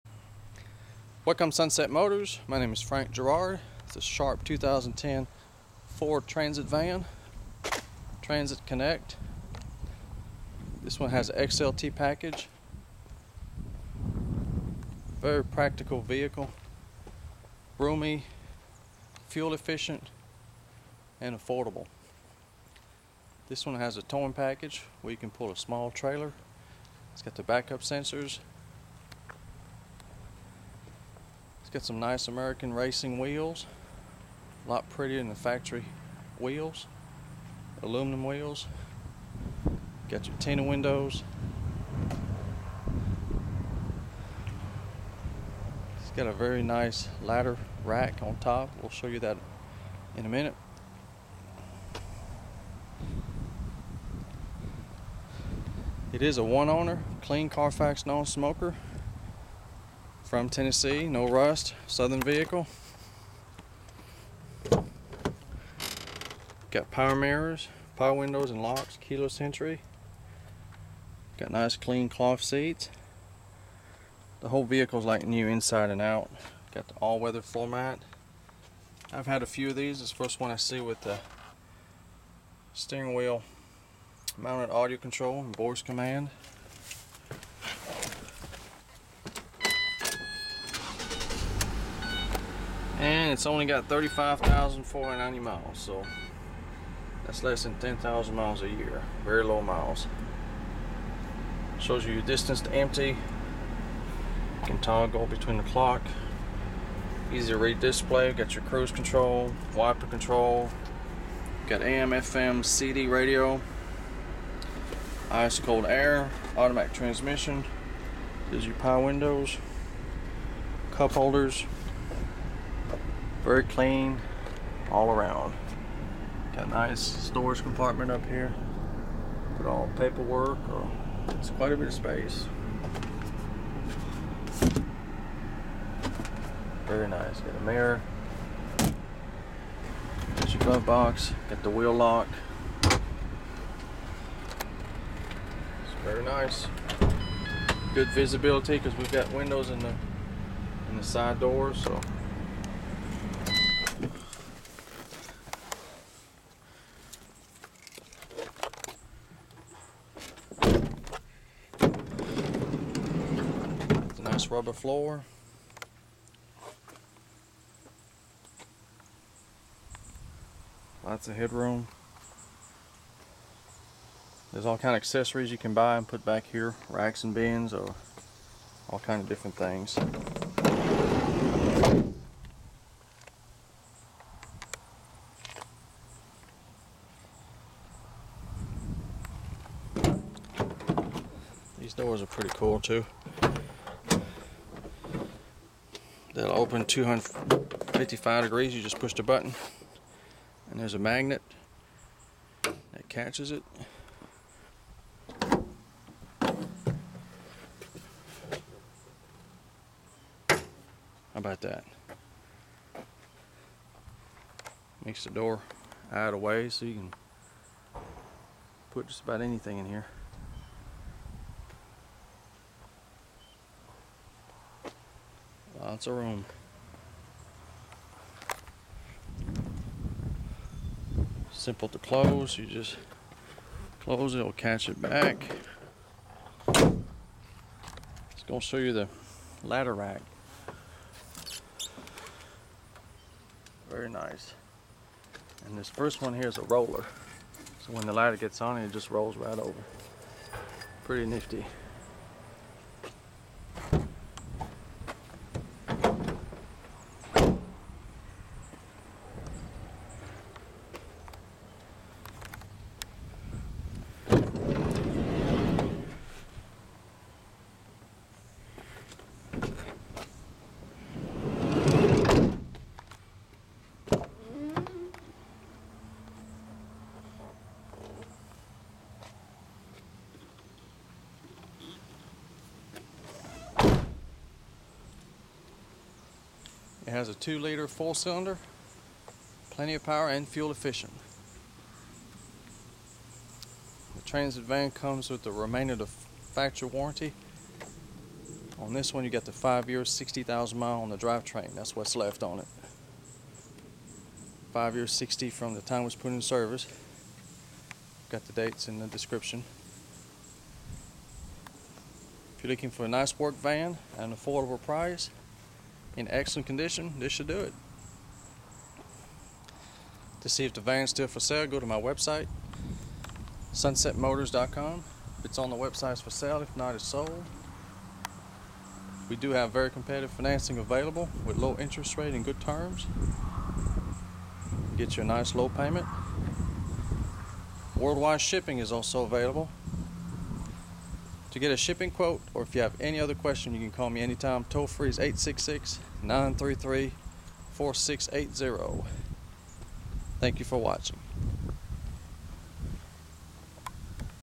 HD VIDEO 2010 FORD TRANSIT CONNECT XLT WHITE USED FOR SALE SEE WWW SUNSETMOTORS COM
FOR MORE INFO AND A VIDEO, 1 OWNER CLEAN CARFAX SOUTHERN 2010 FORD TRANSIT CONNECT CARGO VAN, XLT TRIM PACKAGE, FUEL EFFICIENT 2.0L 4 CYLINDER ENGINE, 22 MPG CITY 25 MPG HWY, AUTOMATIC TRANSMISSION, FRONT WHEEL DRIVE, ONLY 35K MILES !!, UP FITTED WITH HIGH QUALITY ALUTRACK ALUMINUM CARGO RACK FOR YOUR LADDERS OR OTHER ACCESSORIES, AM/FM/CD/AUX RADIO WITH STEERING WHEEL MOUNTED AUDIO CONTROLS INCLUDING VOICE COMMAND, POWER WINDOWS, LOCKS & MIRRORS, KEYLESS ENTRY, 12 VOLT OUTLETS, REAR CARGO 255 DEGREE OPENING DOORS, BACK UP SENSORS AND BUZZER, AMERICAN RACING ALLOY WHEELS, 4 WHEEL ABS BRAKES, ROLL STABILITY CONTROL, DUAL FRONT AIRBAGS, TIRE PRESSURE MONITOR, 15.4 GALLON FUEL TANK, PAYLOAD 1,600 LBS, ALL WEATHER FLOOR MATS, CRUISE CONTROL, 2 SLIDING SIDE DOORS WITH WINDOWS, DUAL REAR WIPERS WITH DEFROST, DELAY WIPERS, LIKE NEW READY FOR WORK, REMAINDER OF FACTORY WARRANTY 03/21/2016 OR 60,000 MILES ON DRIVETRAIN, COMPETITIVE FINANCING AND WORLD WIDE SHIPPING IS AVAILABLE CALL ANYTIME TOLL FREE